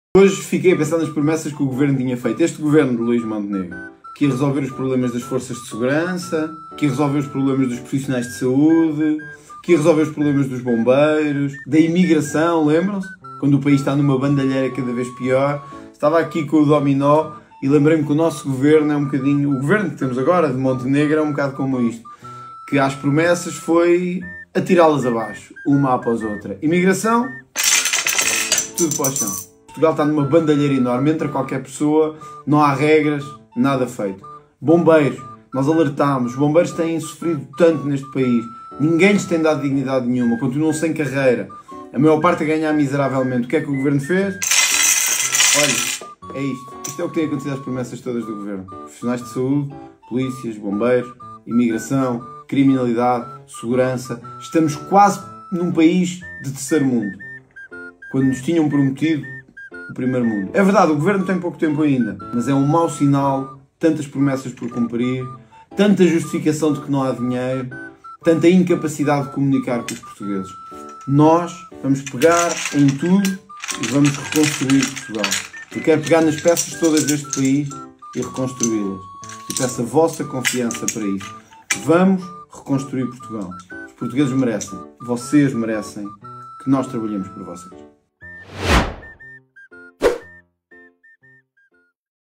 24 PT CH S 13 "This government's promises have been like a castle made of dominoes..." CHEGA Party
Digital library of the European Elections Monitoring Center - EEMC

Election Campaign for the European Parliament 2024

"This government's promises have been like a castle made of dominoes: every day another one falls. They promised everything to everyone and now they don't keep anything promised. Will this Government fall apart?", CHEGA.

24 PT CH S 13

Copyrights:
CHEGA Party